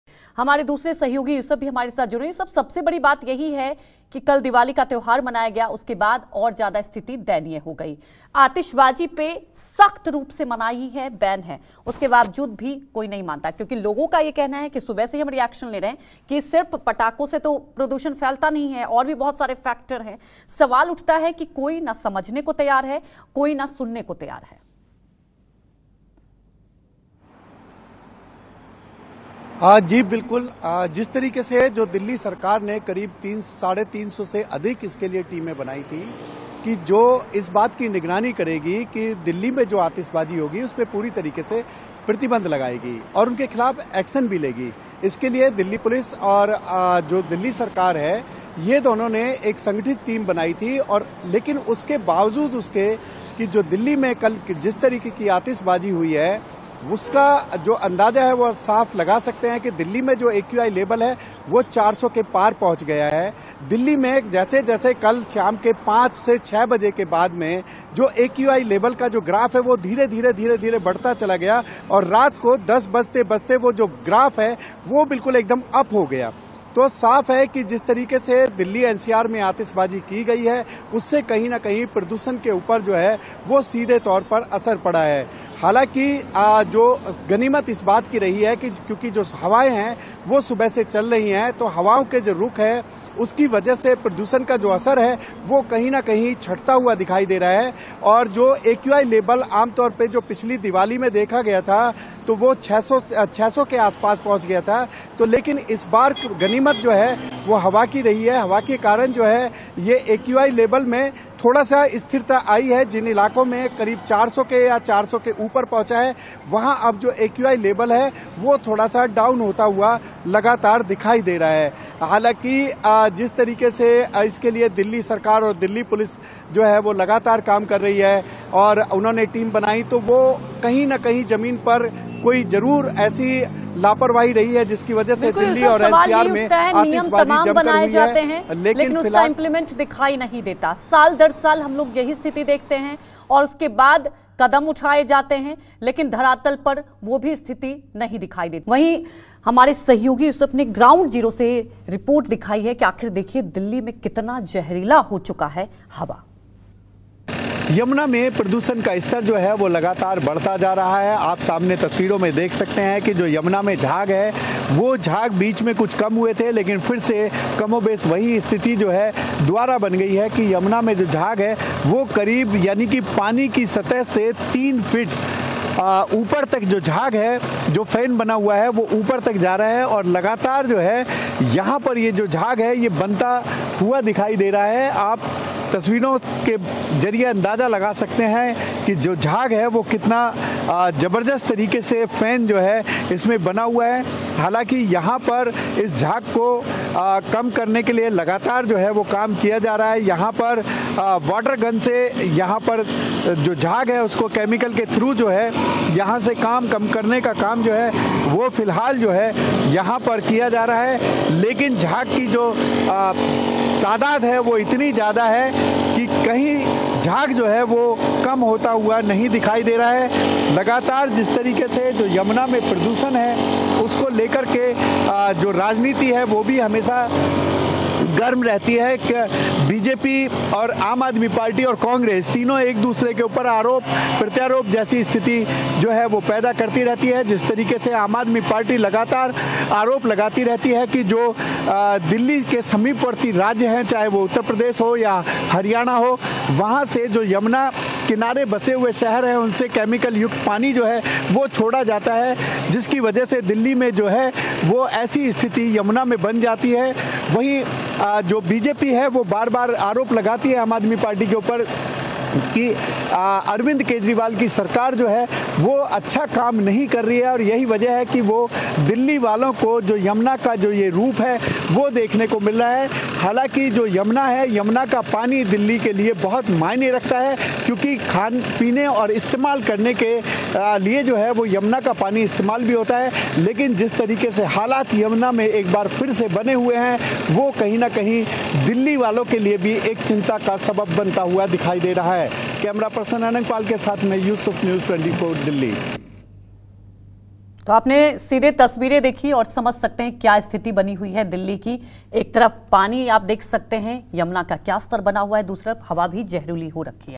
Delhi-NCR में प्रदूषण बढ़ाने वाली ‘दिवाली’…हवा के साथ पानी भी प्रदूषित ! कितना खतरनाक AQI का स्तर ?
Delhi-NCR में प्रदूषण बढ़ाने वाली ‘दिवाली’…हवा के साथ पानी भी प्रदूषित ! कितना खतरनाक हुआ AQI का स्तर ?